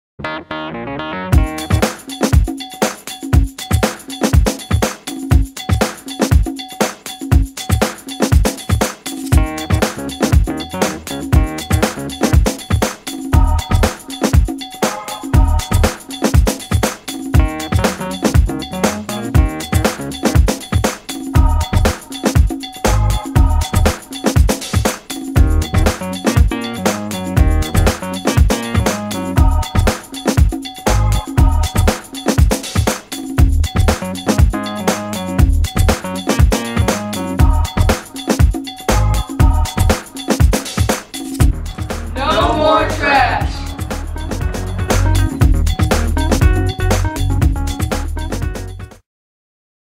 No MOre Trash!
April is Trash Bash month. Help us keep Missouri beautiful!